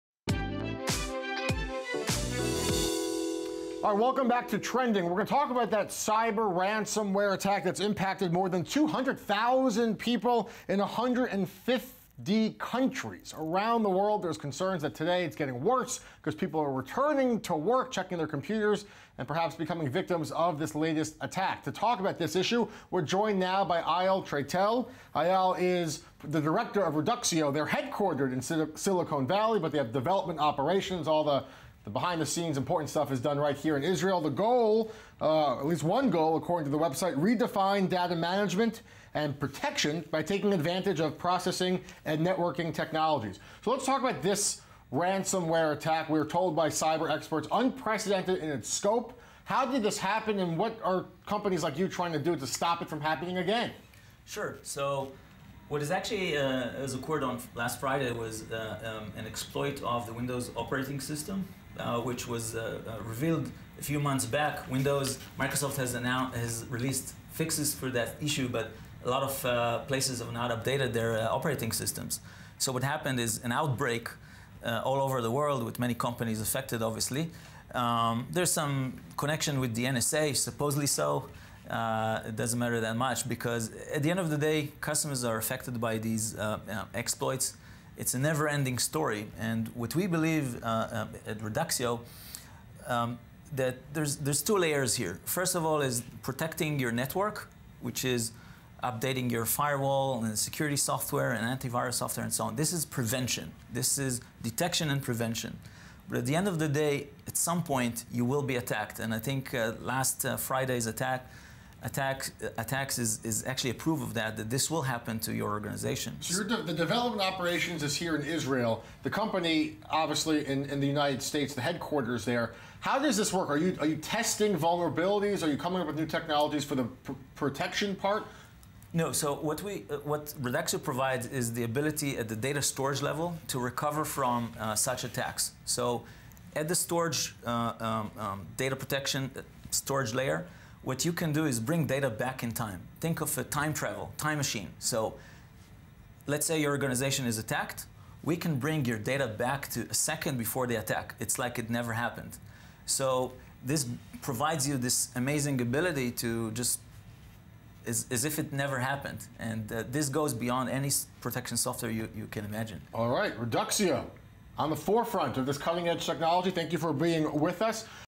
Eyal Traitel on i24 NEWS about Reduxio Storage
Following the last cyber attack, Trailtel talks about detection and prevention of such attacks as well as the ability to go back in time, using Reduxio storage solutions. AKA, the amazing time travel: Recover to any second.

Reduxio is represented in Israel by Consist.